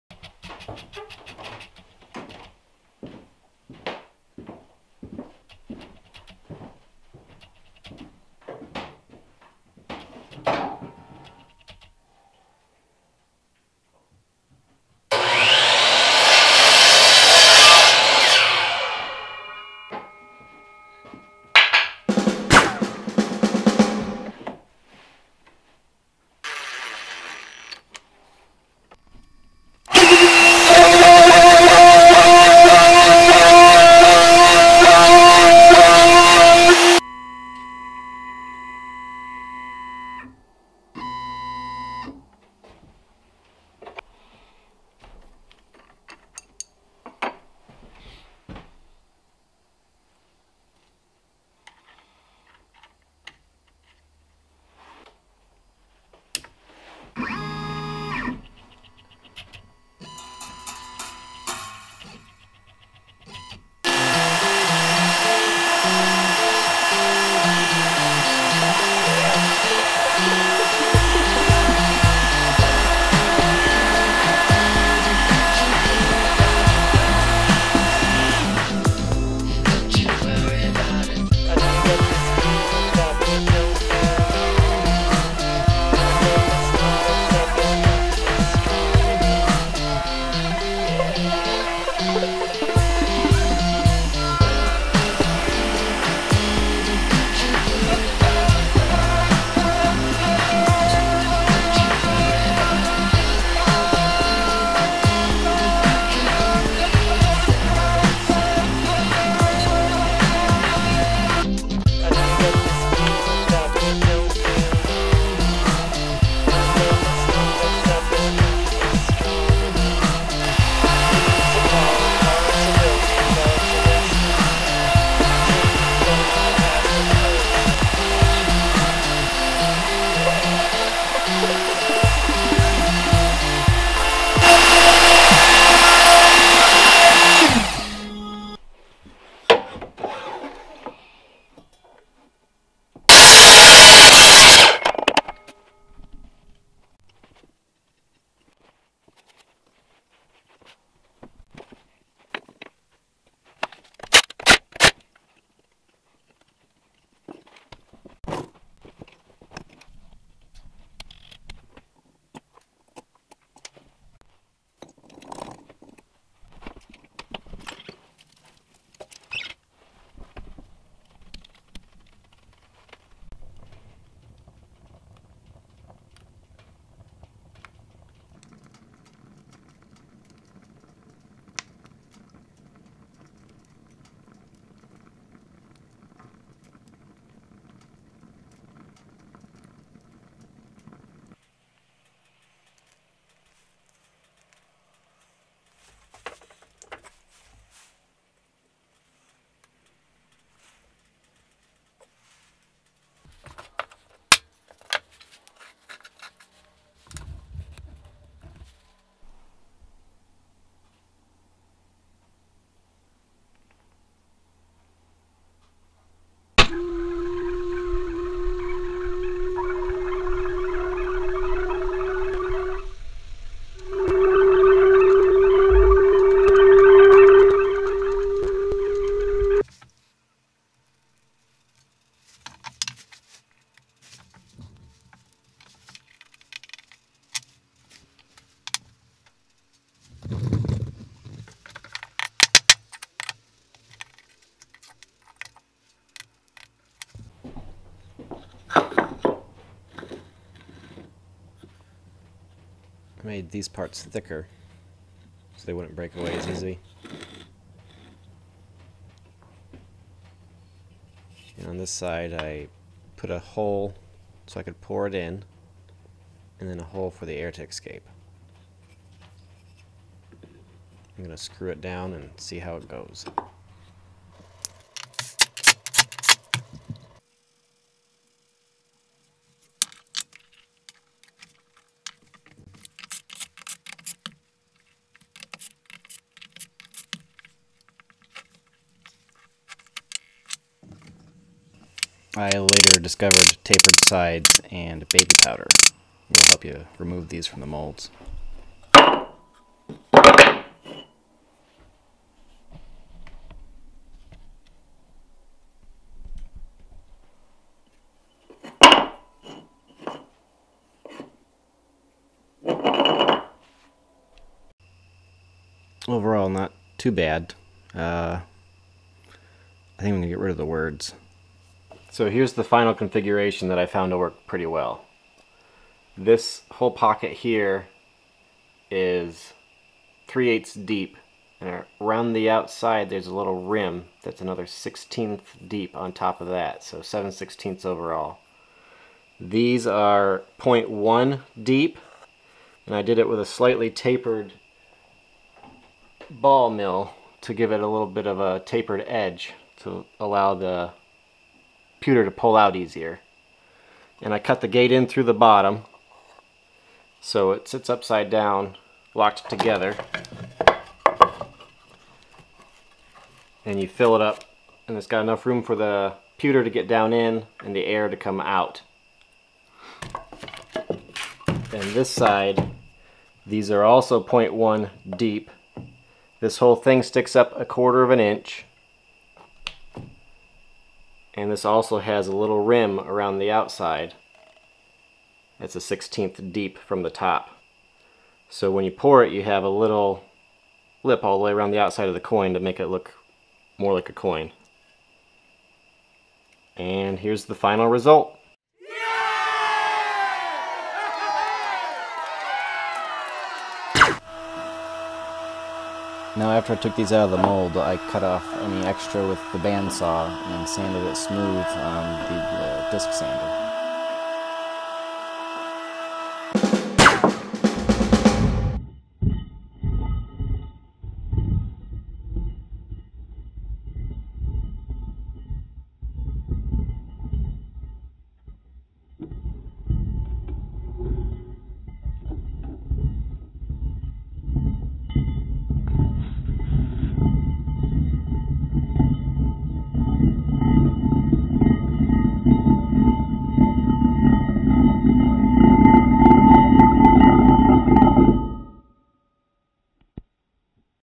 Using Wood to Make Metal Coins
Here is a quick look at some coins I made using wood as the mold. The coins are made from pewter which has a low melting point. I used both Maple and Pine for the molds and had decent luck with both. the mold would only yield about 3 coins before they became damaged.

Tapered Ball Mill
1/16th Endmill